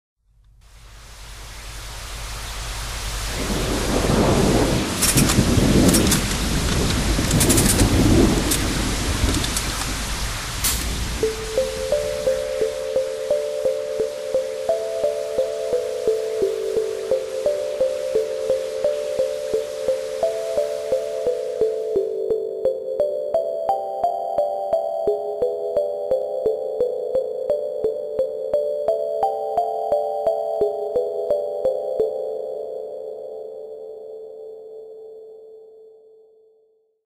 Music box song Sneak Peek (No name yet, vote below) {1 hour of work}
An hour into my work, and this is what I have, Sorry that its such crappy quality :/ It wouldnt let me upload it otherwise